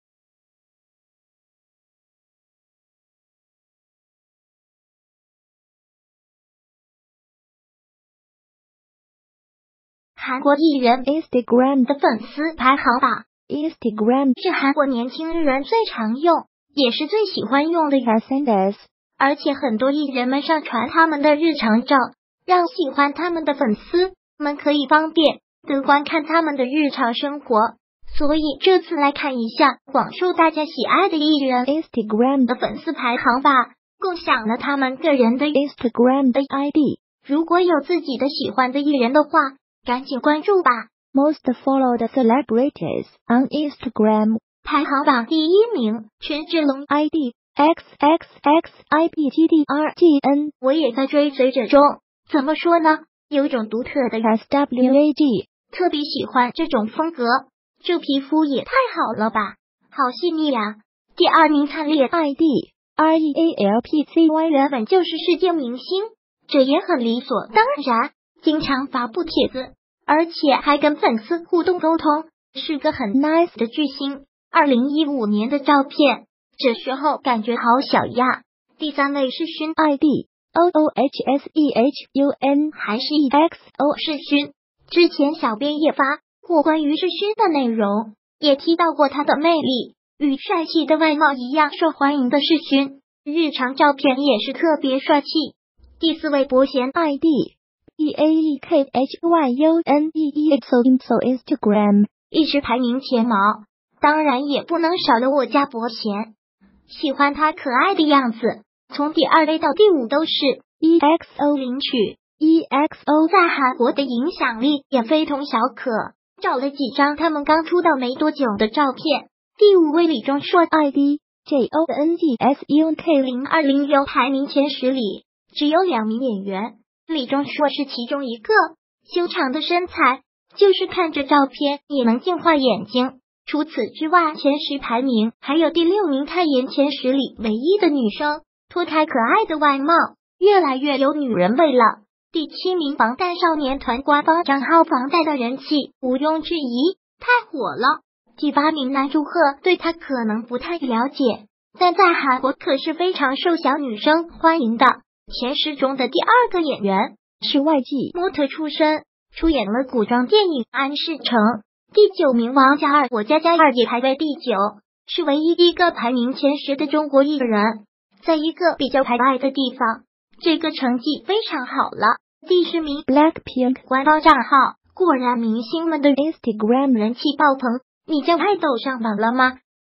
韩国艺人Instagram的粉丝排行榜，权志龙排名第一位
韩国艺人Instagram的粉丝排行榜。Instagram是韩国年轻人最常用也是最喜欢用的SNS。而且很多艺人们上传他们的日常照, 让喜欢他们的粉丝们可以方便地观看他们的日常生活。所以这次来看一下广受大家喜爱的艺人Instagram的粉丝排行吧。共享了他们个人的Instagram的ID。如果有自己的喜欢的艺人的话, 赶紧关注吧！Most followed celebrities on Instagram 排行榜第一名 权志龙ID： xxxibgdrgn我也在追随者中, 怎么说呢，有种独特的SWAG！特别喜欢这种风格。这皮肤也太好了吧！好细腻呀! ! !第二名 灿烈ID：real__pcy原本就是世界明星, 这也很理所当然。经常发布帖子, 而且还跟粉丝互动沟通，是个很nice的巨星。2015年的照片，这时候感觉好小呀。第三位 世勋ID：oohsehun还是exo世勋，之前小编也发过关于世勋的内容。也提到过他的魅力。与帅气的外貌一样受欢迎的世勋，日常照片也是特别帅气。第四位 伯贤ID：baekhyunee_exoExo instagram，一直排名前茅。当然也不能少了我家伯贤！喜欢他可爱的